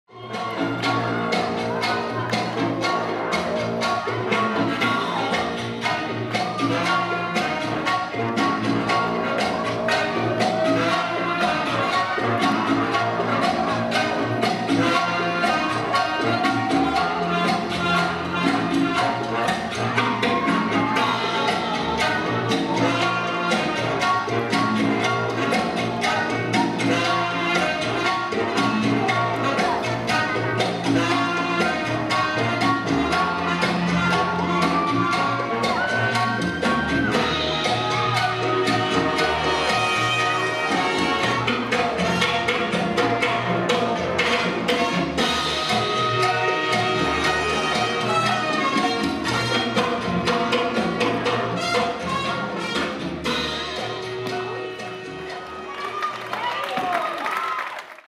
Manolo Borelli - Rosaria Sollo Maria | Italian National Ch. 2021 IDF | U16 LAT - F C
2021 Italian National Champioships IDF.
Event ✨ IDF National Italian Champ. 2021 / @idf_italiandancefederation